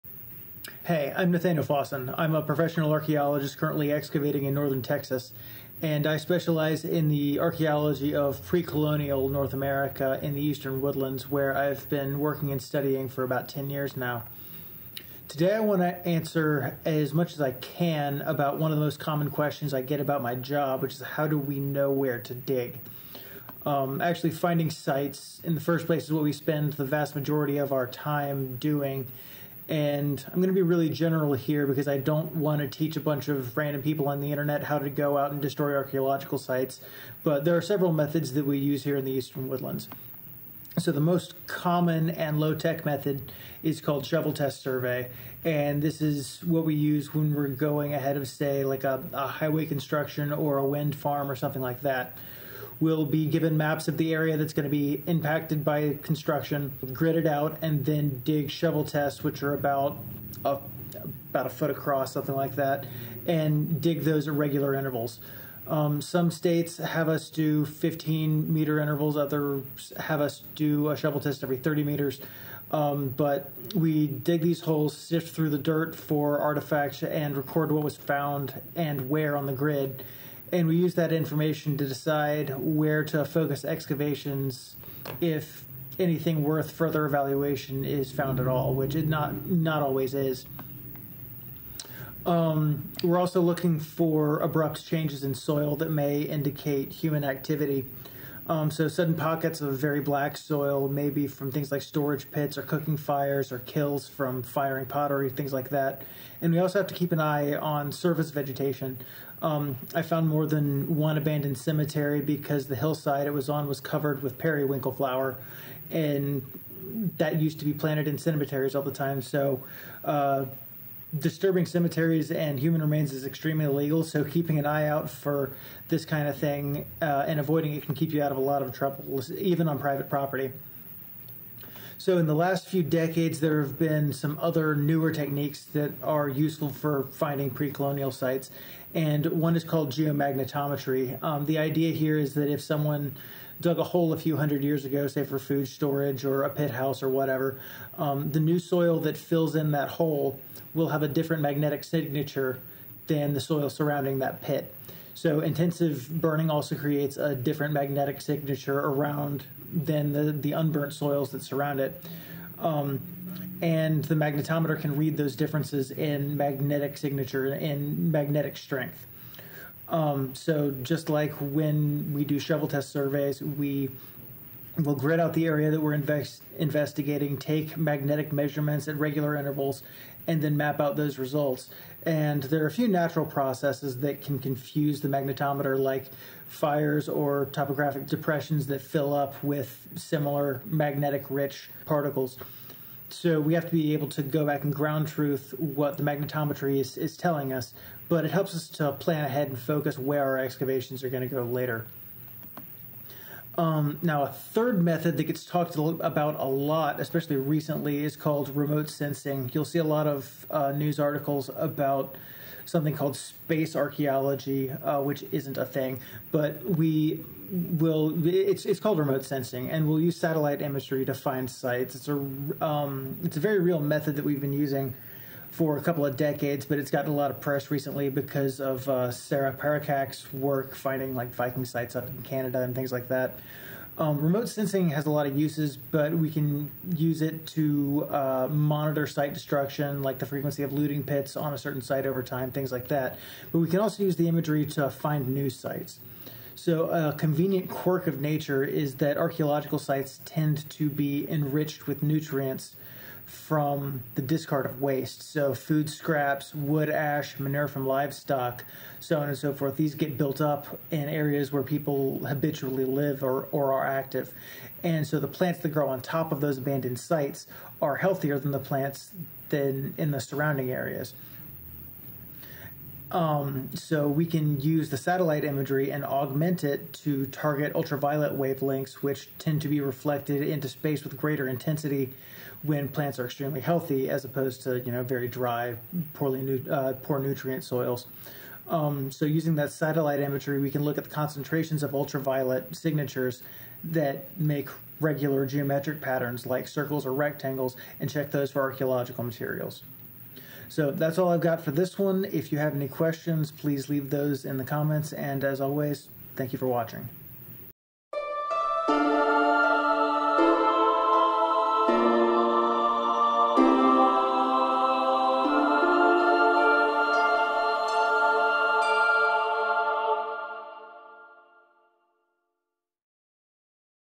How Archaeologists Know Where to Dig: Shovels, Magnets and Satellites.
Archaeologists use a combination of high-tech and shovel-and-screen methods to locate archaeological sites. I describe a few methods that we use here.

Images and References
Jera R. Davis, Chester A. Walker, and John H. Blitz 2015
Remote Sensing as Community Settlement Analysis at Moundville.
American Antiquity

Fiona Marshal et al 2018
Ancient herders enriched and restructured African grasslands.
Nature

Jesse Casana 2015
Satellite Imagery-Based Analysis of Archaeological Looting in Syria.
Near Eastern Archaeology